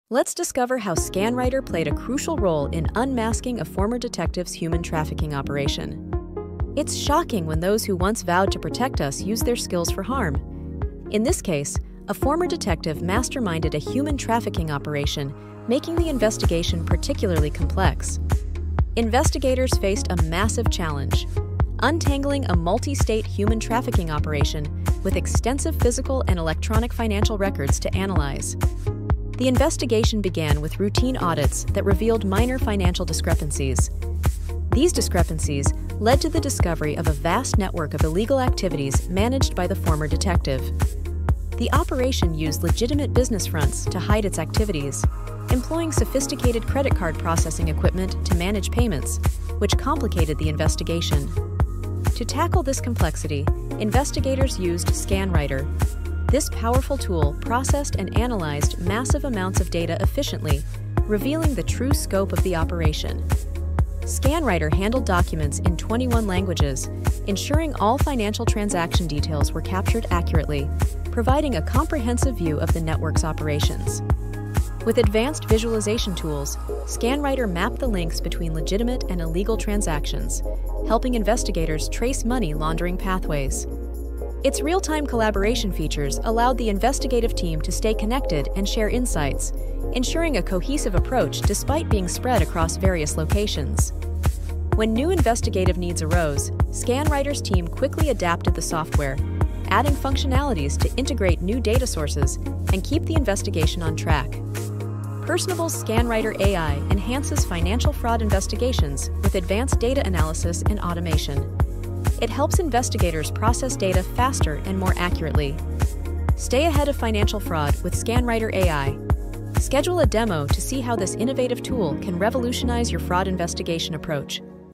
ScanWriter’s Role in Unmasking a Former Detective’s Human Trafficking Operation
Discover how ScanWriter's advanced data entry automation and visualization capabilities were instrumental in unmasking a former detective’s human trafficking operation.

This high-profile investigation involved unraveling a complex web of deceit spread across multiple states.

ScanWriter processed thousands of pages of financial documents and analyzed tens of thousands of transactions, breaking language barriers and providing crucial visualizations.

Learn how ScanWriter’s real-time collaboration and rapid adaptability ensured a coordinated and efficient investigative approach.

See why ScanWriter is trusted by investigators nationwide and how it can revolutionize your fraud detection efforts.

Schedule your free demo today!